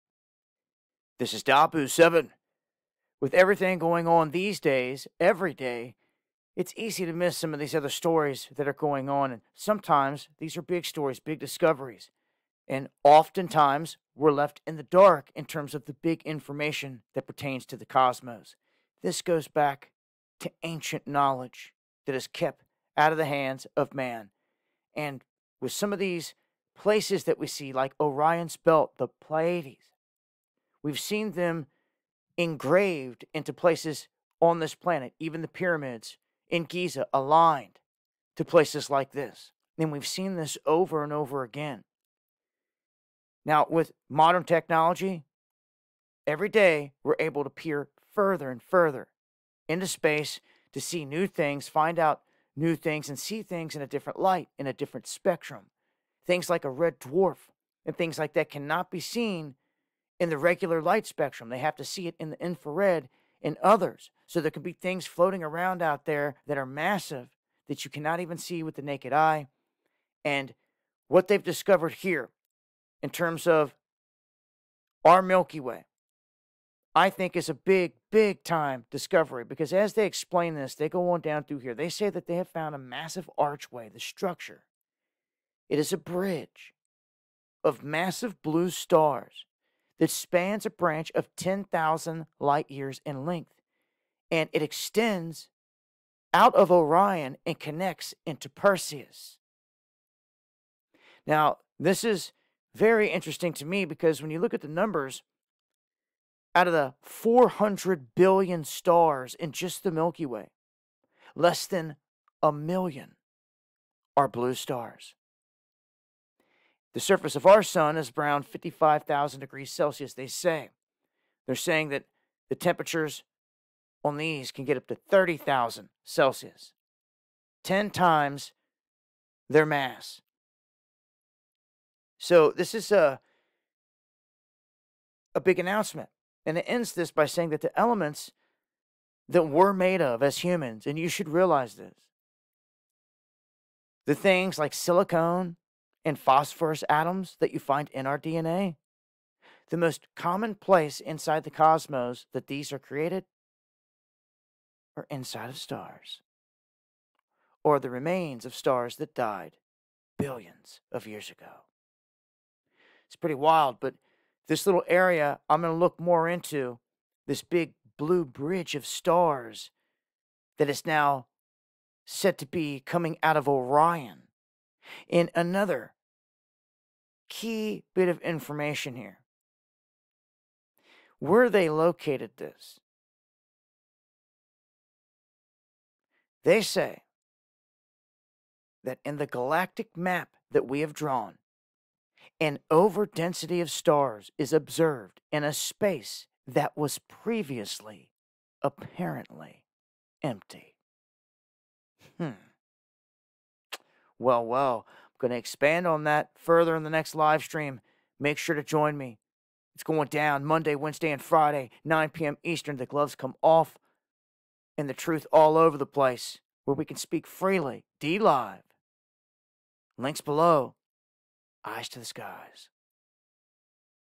Massive Hidden Structure Made of Rare Blue Stars Is Discovered In The Milky Way
Spanish astronomers say they have completed the most accurate picture yet of the Milky Way and have discovered a huge new hidden structure made up of massive blue stars spanning 10,000 light-years.
Astronomers had already known that the Milky Way consists of great spiral arms of stars, but the scientists from Spain’s Centro de Astrobiología (CAB) put together the most detailed map yet of the Orion arm, where our solar system is, along with those of Perseus at the outer edge of the Milky Way, and Sagittarius, located towards the center of our galaxy.-